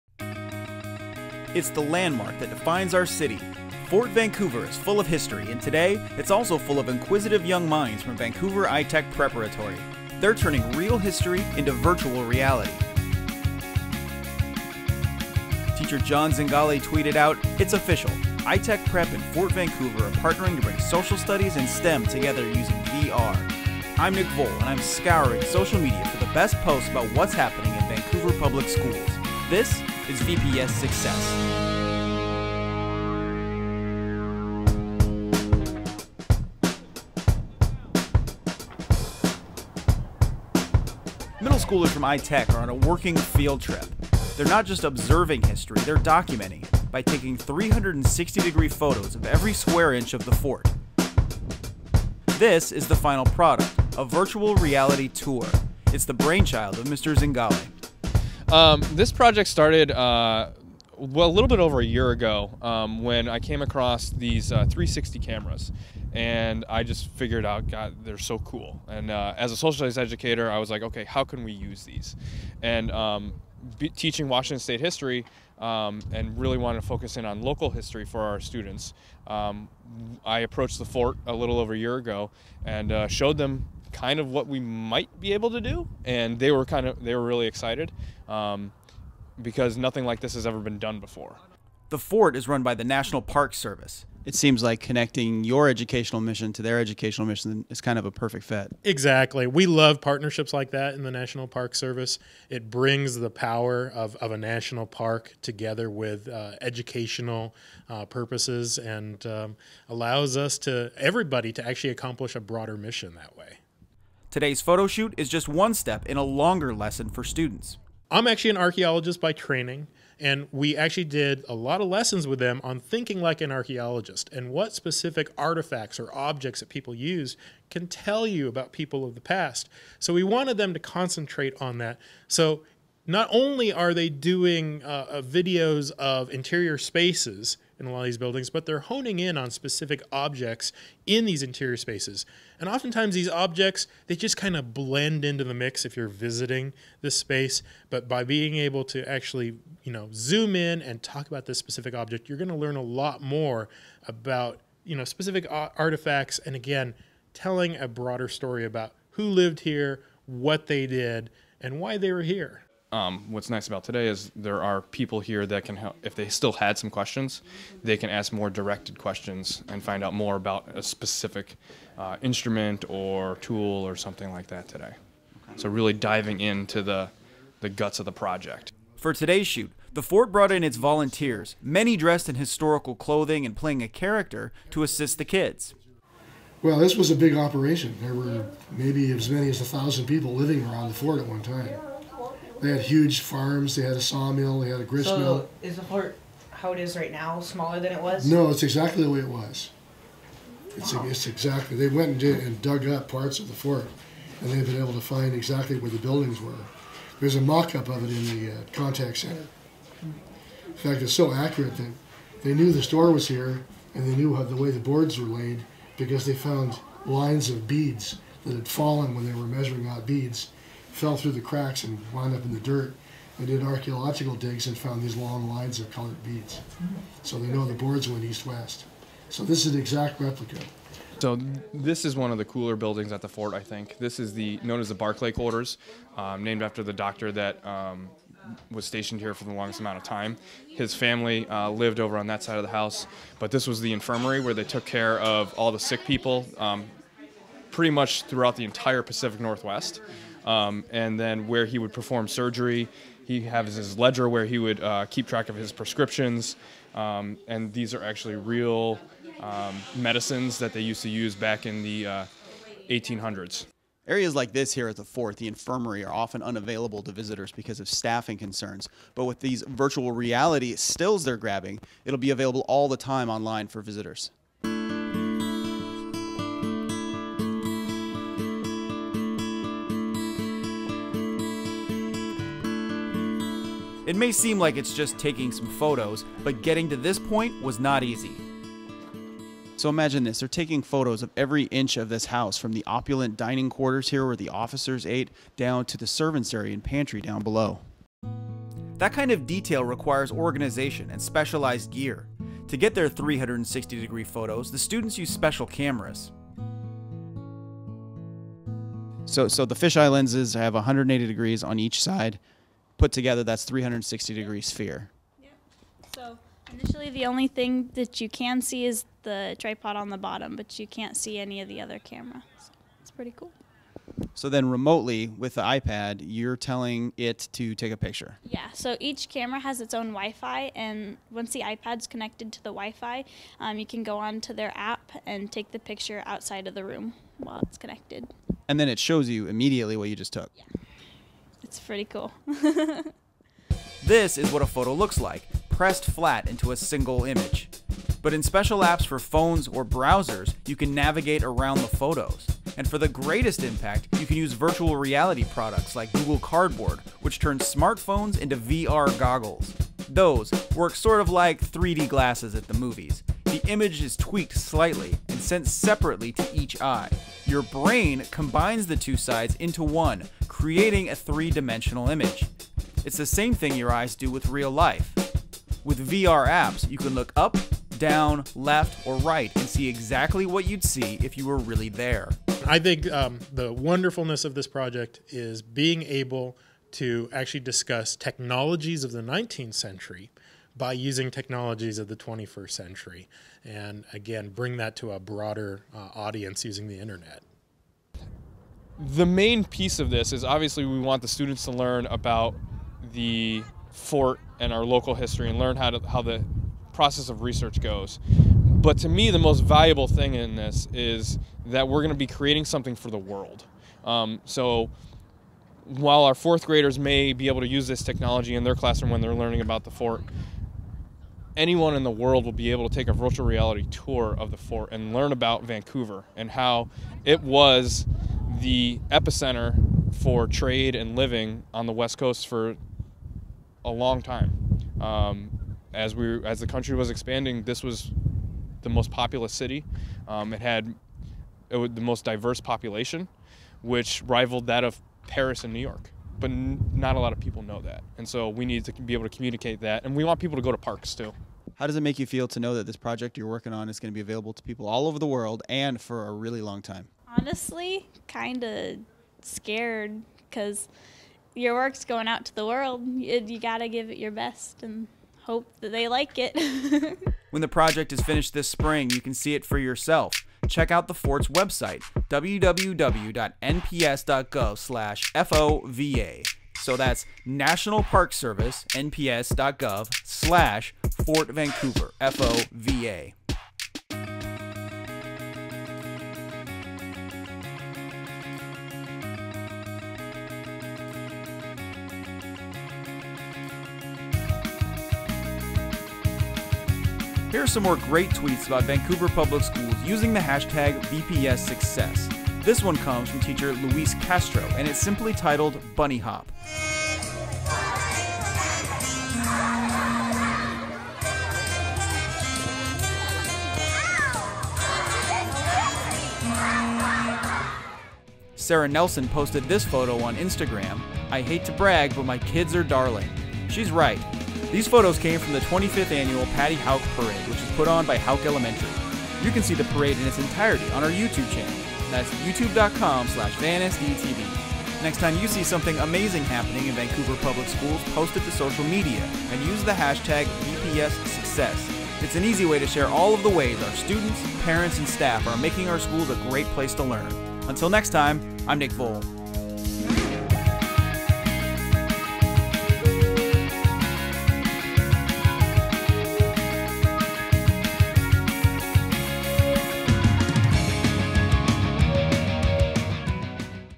VPS Success - Virtual Reality at Fort Vancouver
In this episode, Nick tags along with students from Vancouver iTech Preparatory as they grab 360-degree images for virtual reality tours of the historic Fort Vancouver.  Host: Nick Voll